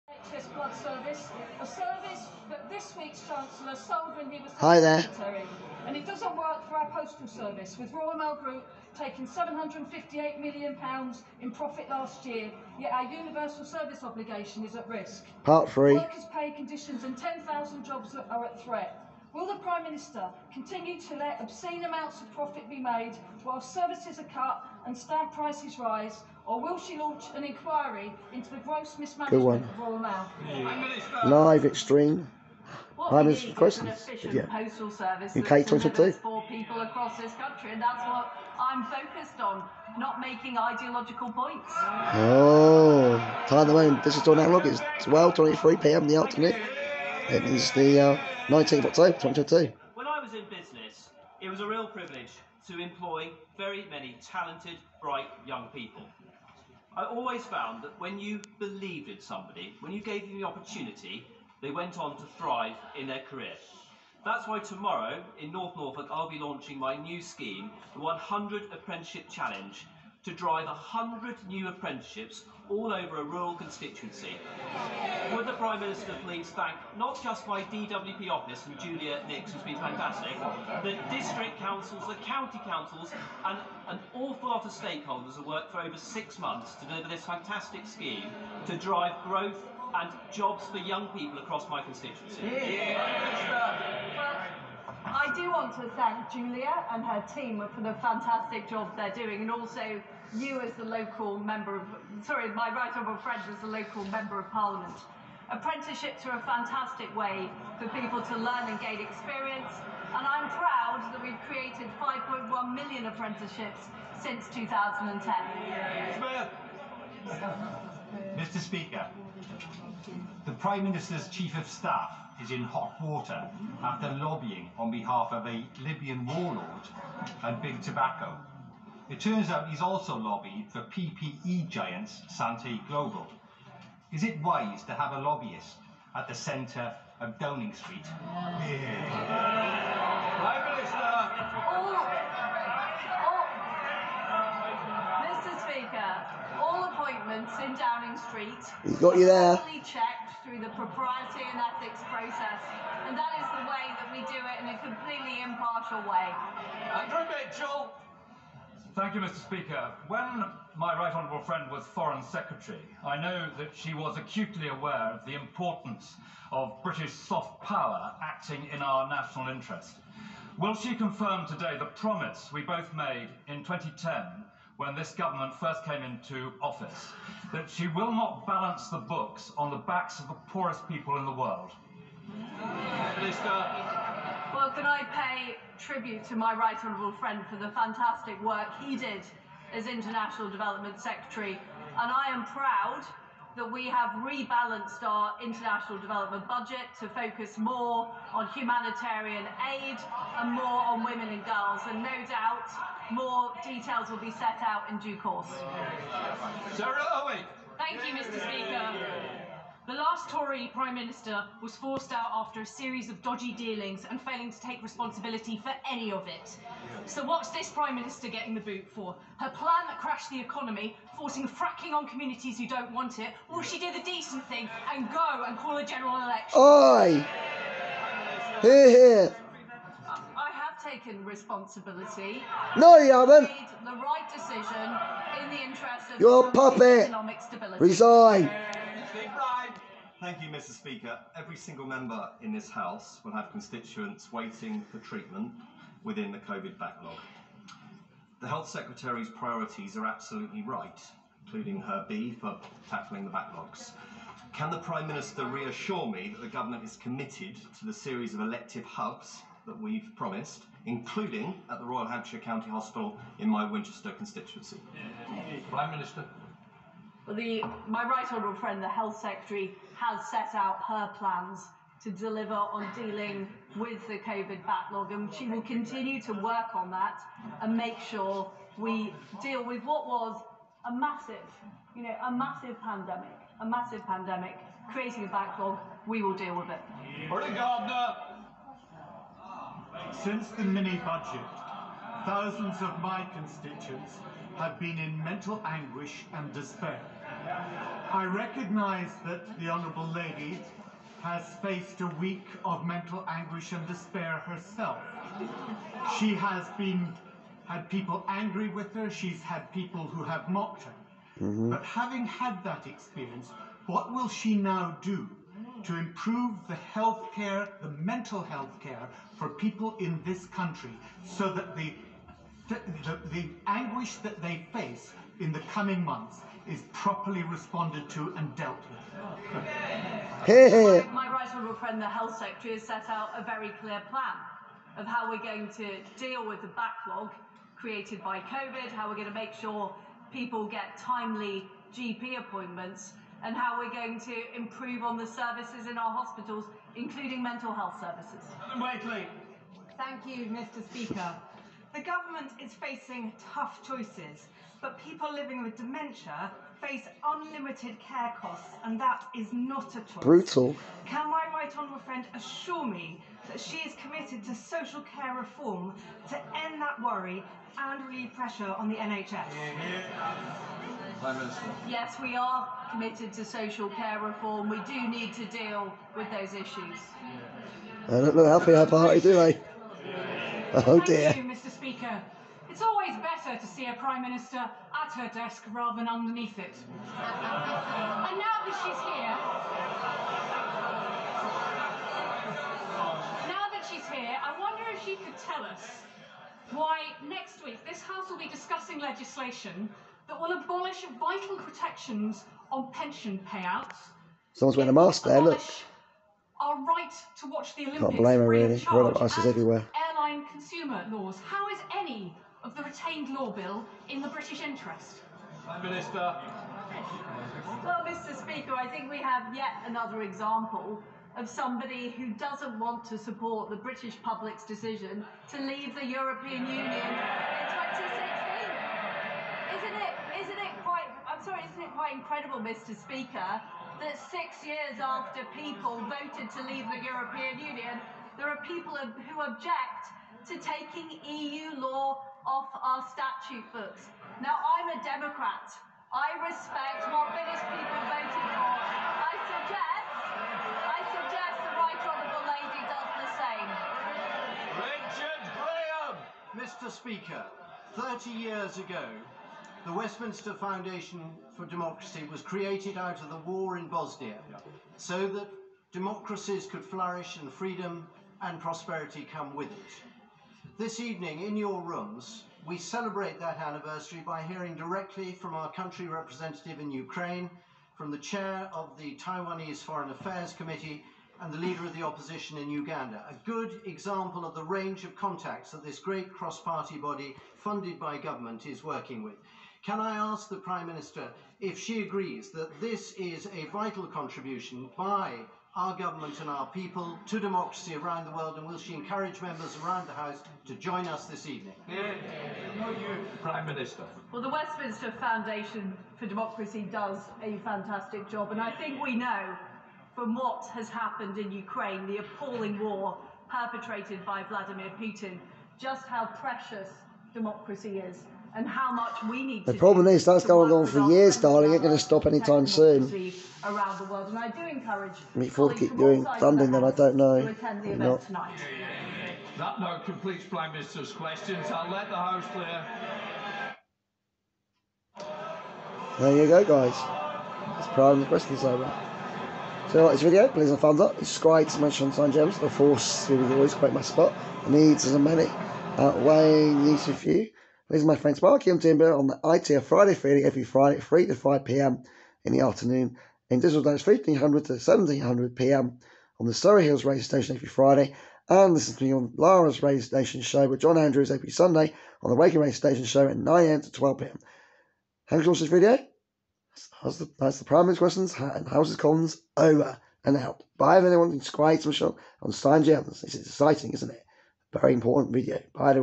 LIVE PART 3 VERY EXTREME PRIME MINISTER'S QUESTIONS HOUSES OF COMMONS UK2022.📜⚔️🛡️🤥🤥🚨👺👍
TALK ABOUT FRACKING IN THE UK IN THE FUTURE FROM THIS  PRIME MINISTER LIZ TRUSS. AND HOUSING FOR THE FUTURE ACROSS THE UK AS WELL. I DON'T TRUST LIZ TRUST DO YOU TRUST HER THE HOMEWRECKER OF ECONOMY OF THE UNITED KINGDOM. 🌪️🌪️😷📜⚔️🛡️🤥🚨👺👍